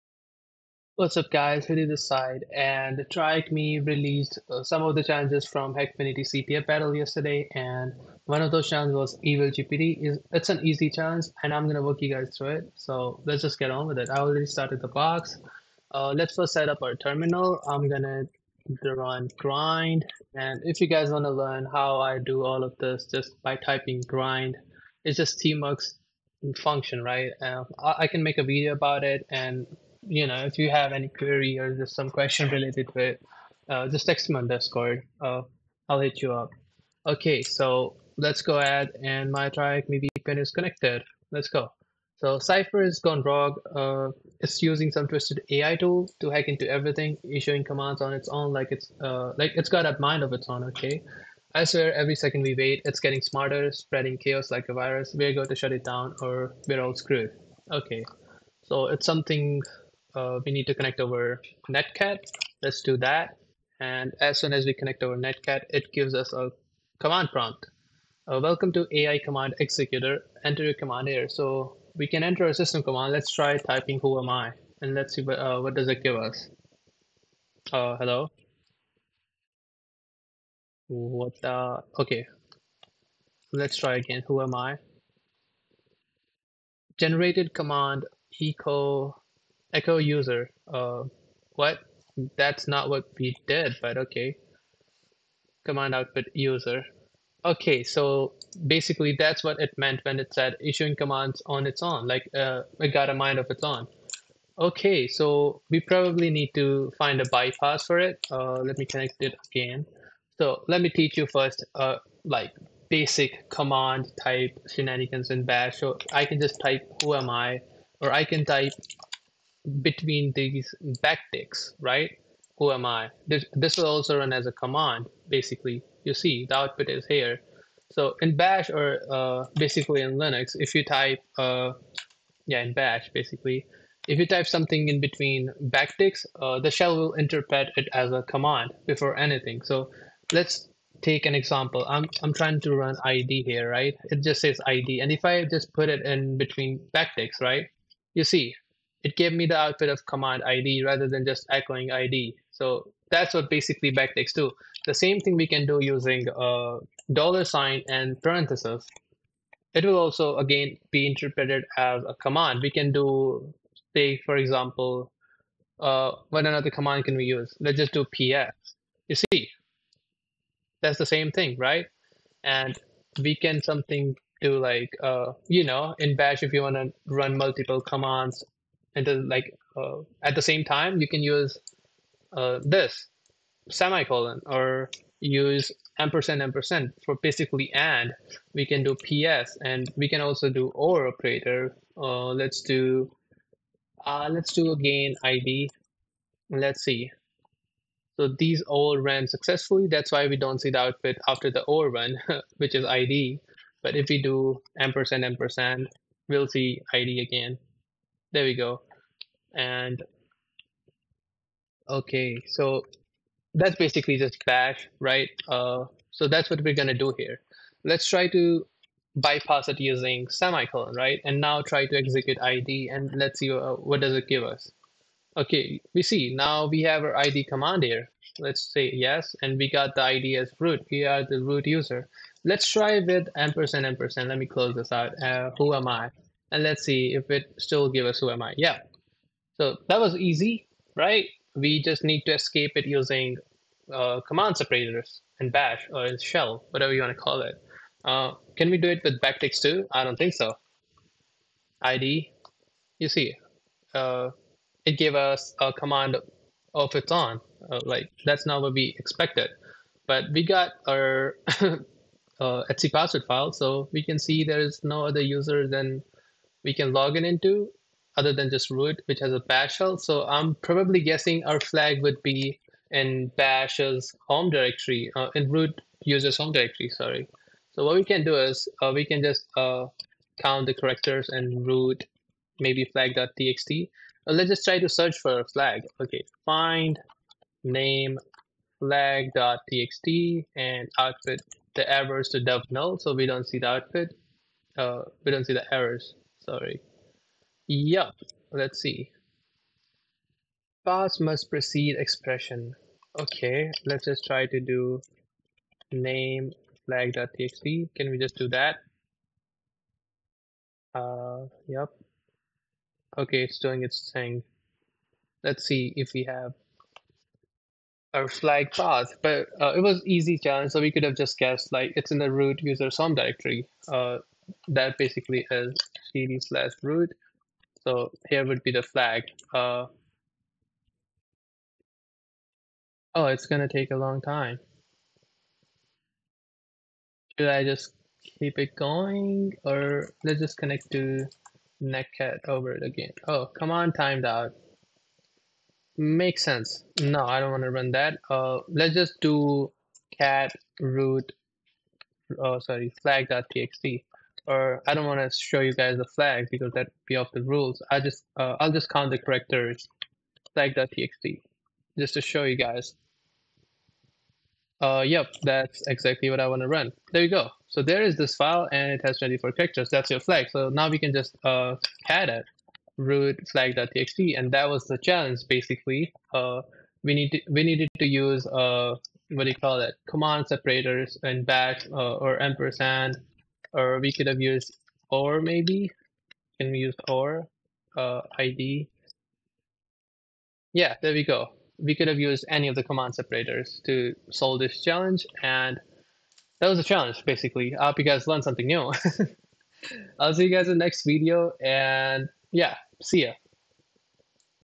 Evil-GPT TryHackMe Walkthrough | Easy
In this video, we are solving TryHackMe's Hackfinity Battle CTF challenge Evil-GPT. Here we will see how to bypass an AI Command Executor and get our flag.txt. Hope you guys learn something new, have a good day 🙏🚀❤️

Support This Tramp!